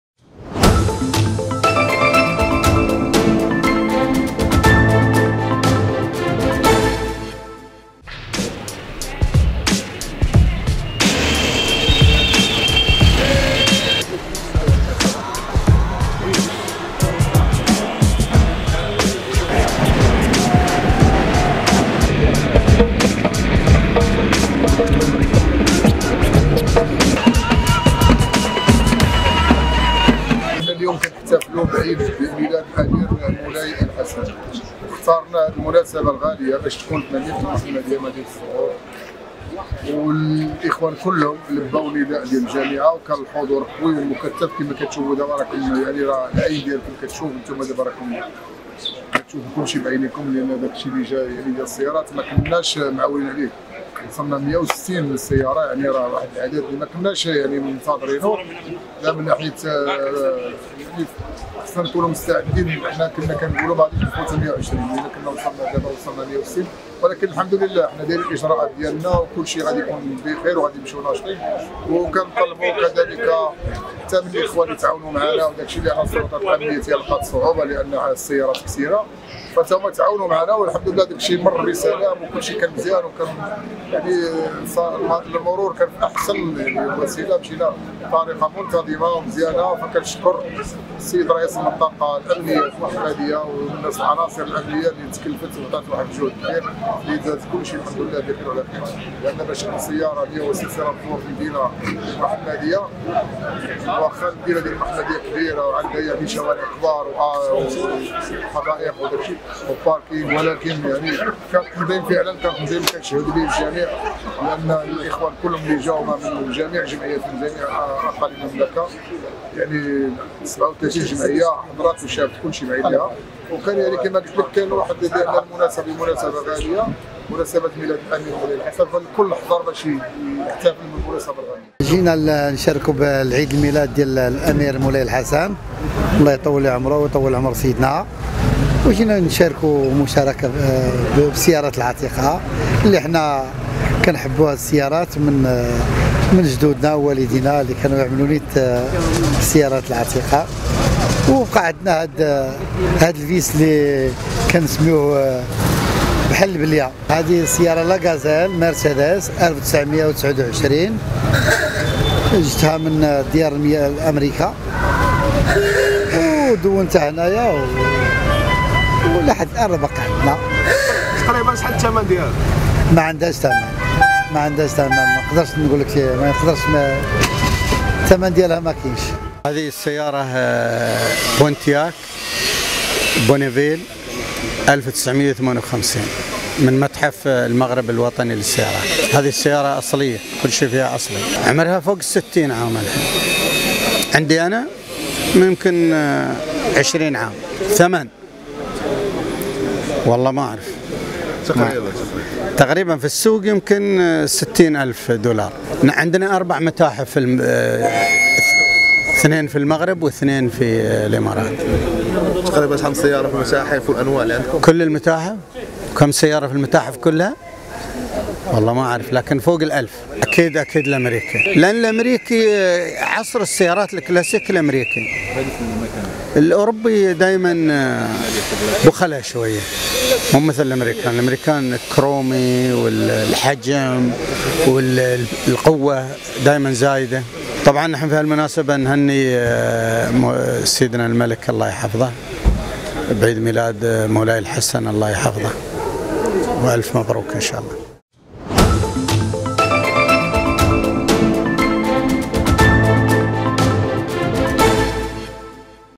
المحمدية.. هذه أبرز لحظات لحاق السيارات العتيقة
في هذا الفيديو ستقف عبى أبرز لحظات لحاق السيارات العتيقة، في مدينة المحمدية وهي سباق تشارك فيه مجموعة من السيارات العتيقة